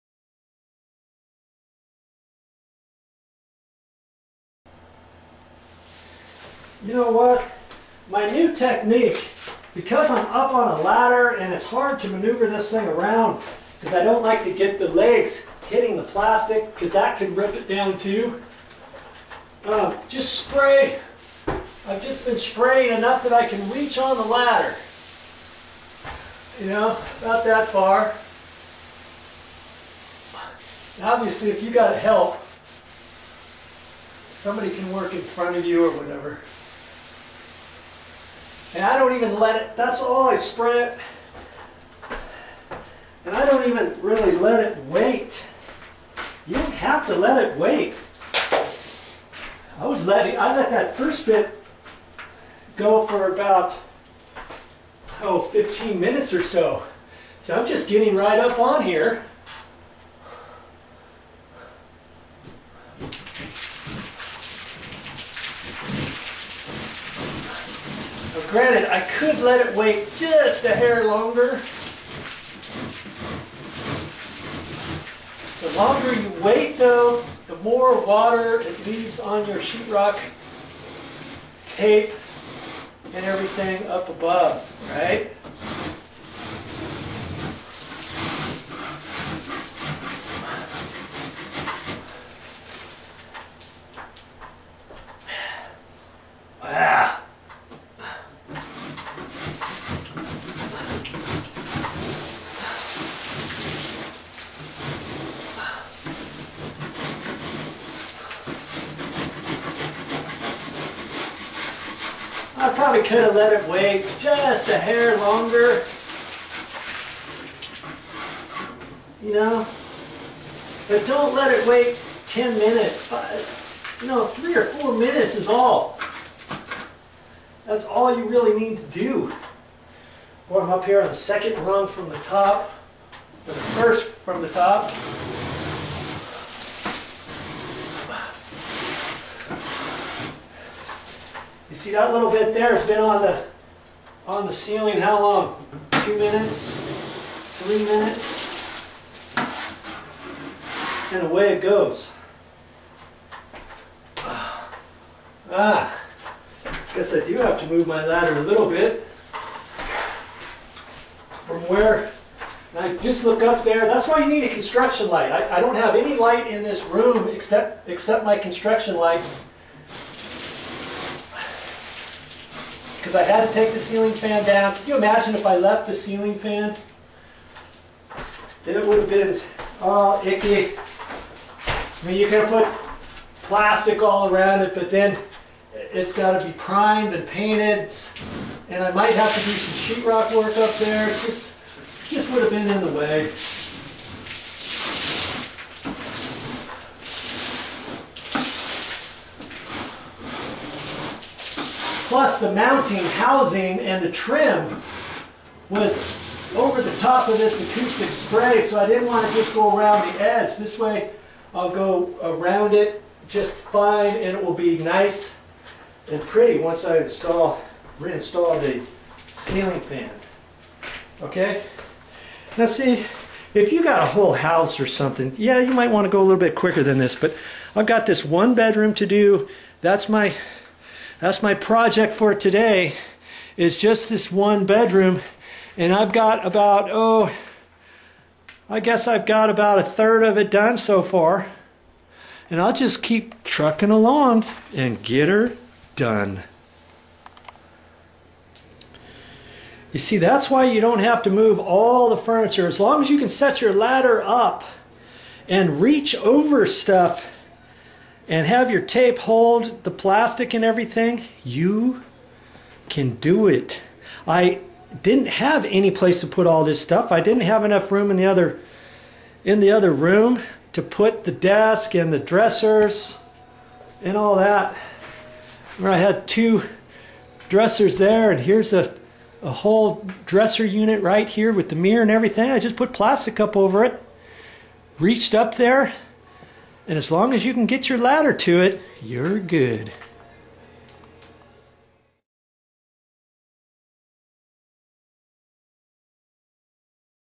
How to remove a popcorn ceiling...Part 6
How to remove a popcorn ceiling (Part 6) is easy...when you know how. Was your home built
in the 70's or 80's? Chances are your ceilings are the popcorn ceilings too. The popcorn ceilings probably have
dis-colored over the years & may have been patched a time or two & look pretty dirty in places...or just overall.
You really wished you didn't have the popcorn ceilings anymore. You really want to have them removed but you heard it
could get pretty pricey to have someone come in & do all the work. especially since you live there too! What a
project & no wonder the quotes came in more than you expected! But did you know you could remove the popcorn ceilings
yourself & save all that dough?!! Yes. Good thing "joe" just went through this nightmare project. & he made videos
of his popcorn ceiling project removal too! Way to go "joe"!! So if "joe" can do it. Yes I know you know that you
can do it too! This episode "joe" will be discussing & showing how to actually remove the popcorn ceiling. Yes. he's doing it!